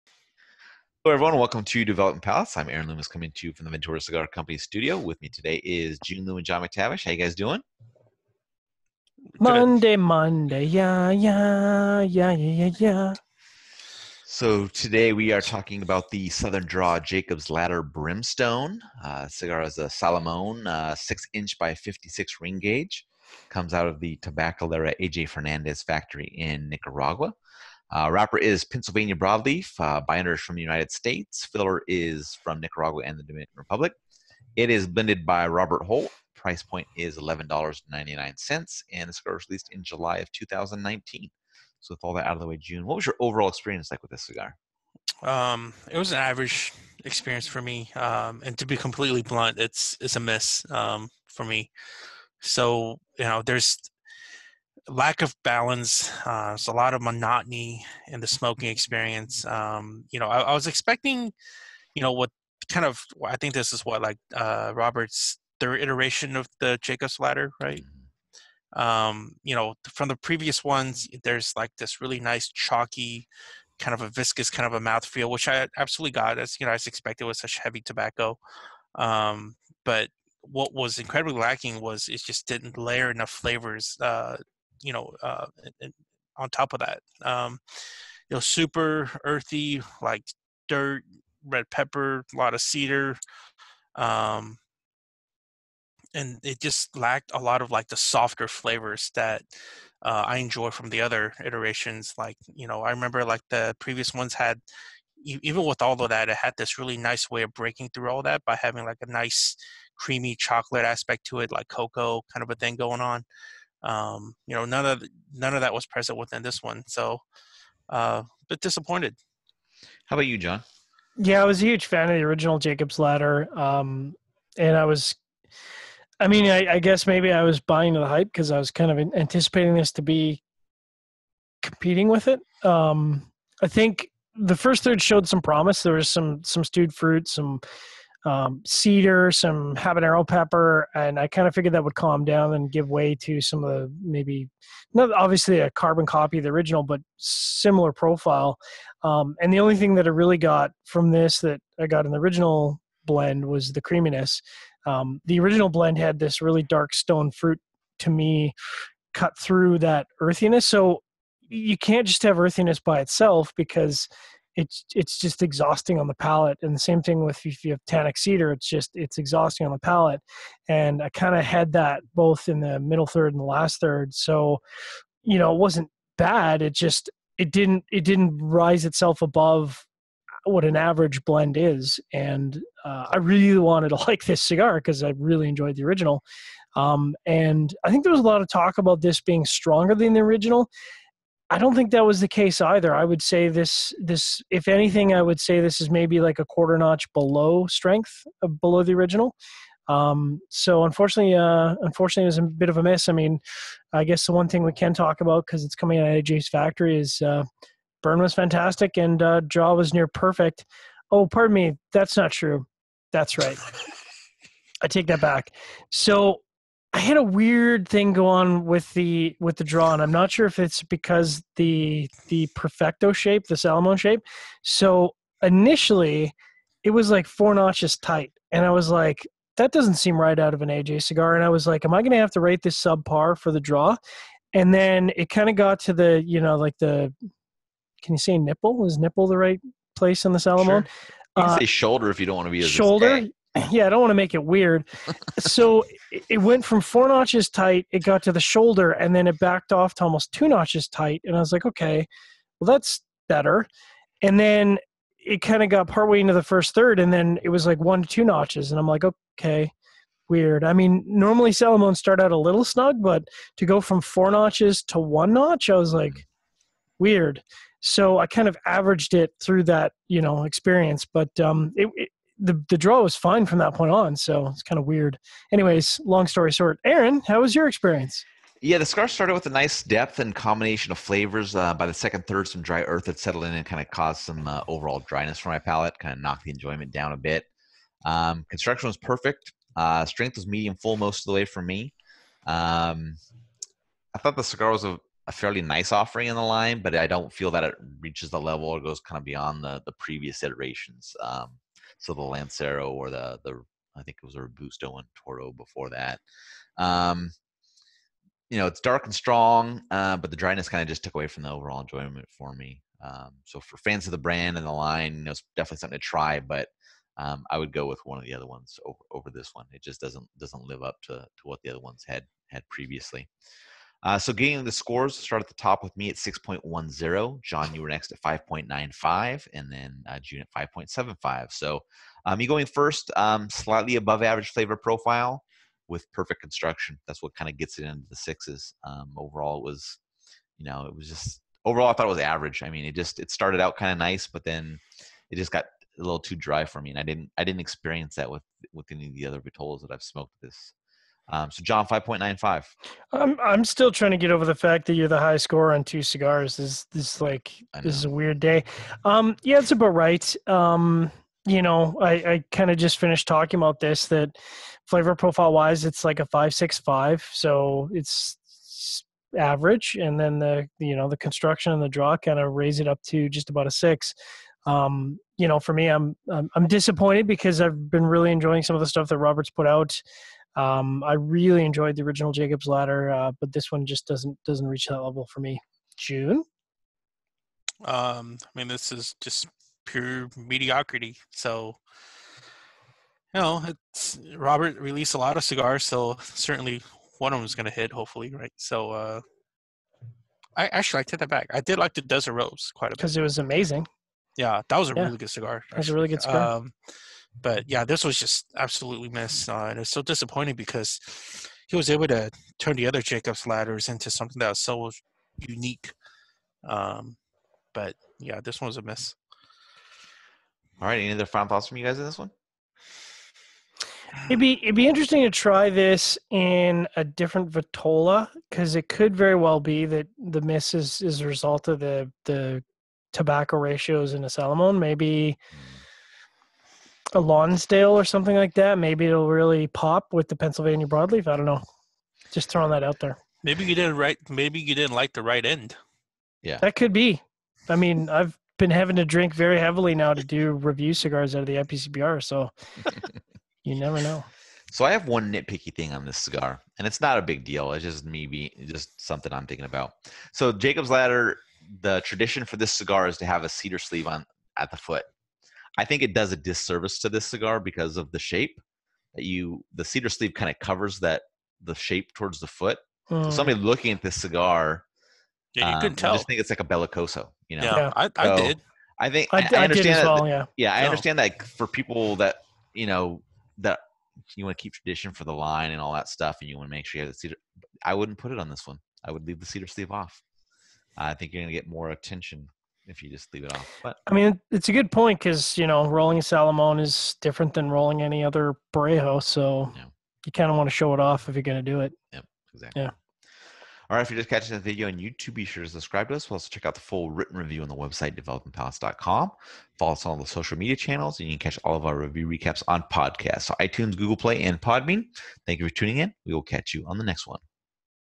Team Review Recap: Southern Draw Jacobs Ladder Brimstone
Jiunn, John and Aaron discuss their review experience with the Southern Draw Jacobs Ladder Brimstone